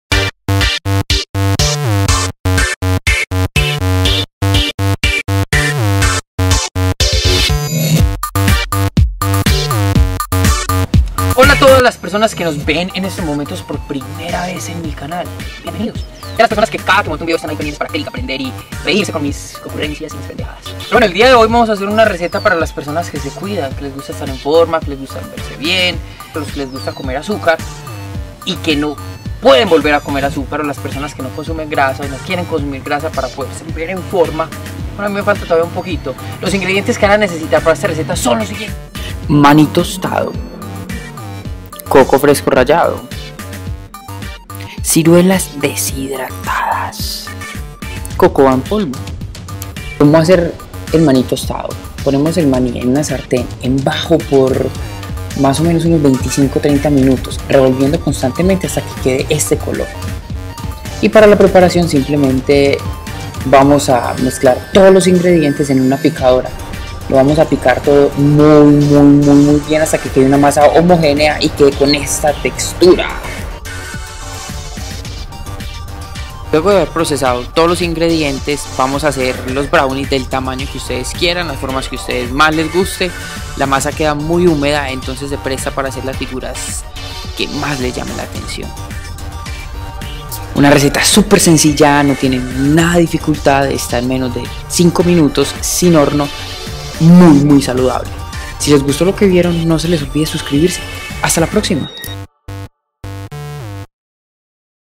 BROWNIE SIN AZUCAR, SIN GLUTEN, SIN HUEVO SIN HORNO Y SIN GRASAS SATURADAS BY JuanDaimond
Ingredientes:

1.procece todos los ingredientes en picadora o licue  hasta que formen una pasta homogénea y suave.
2.arme los brownies del tamaño y forma que desee.

Nota: si les hace falta mas dulce se puede usar sobres de endulzante.